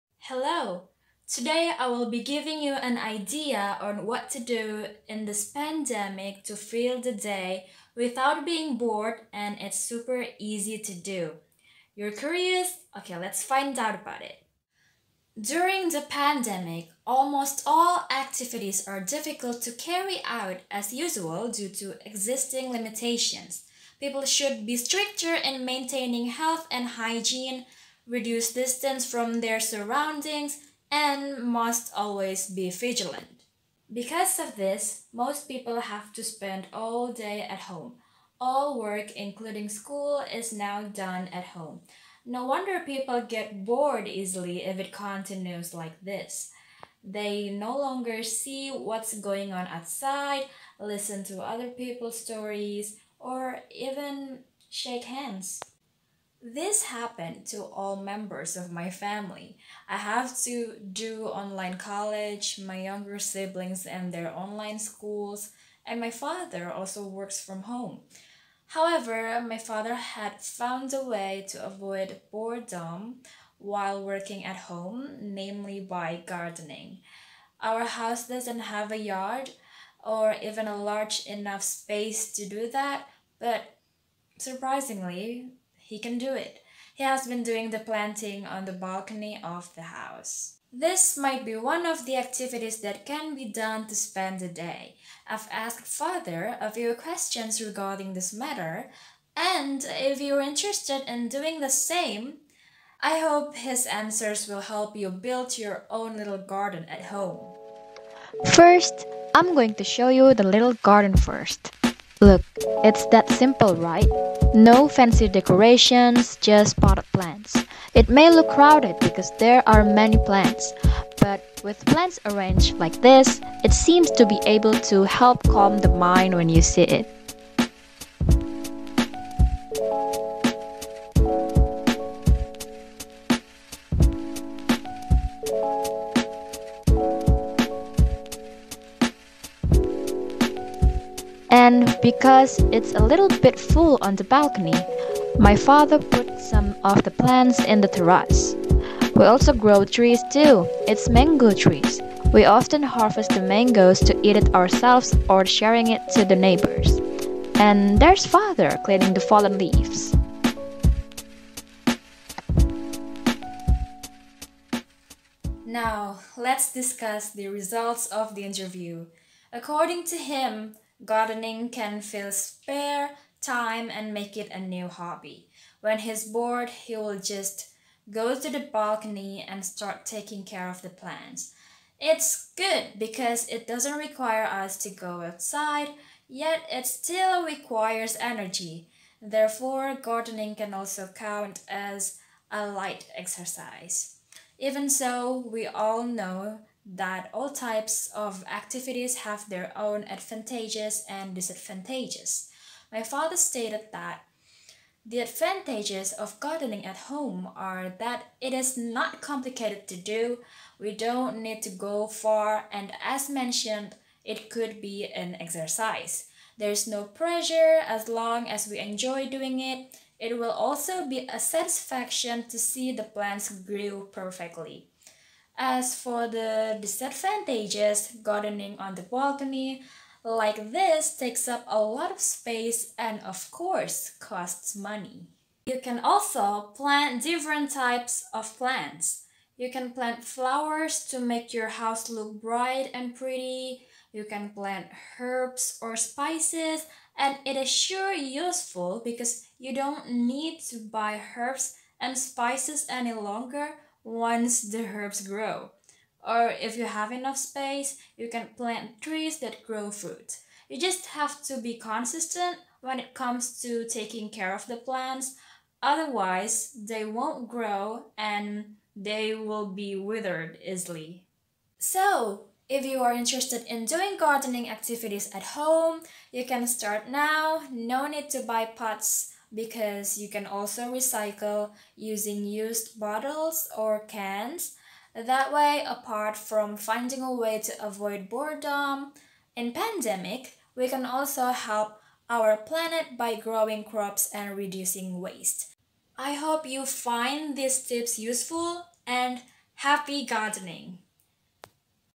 Gardening at Home
During this pandemic, gardening can be a solution for people who are often bored because of working from home.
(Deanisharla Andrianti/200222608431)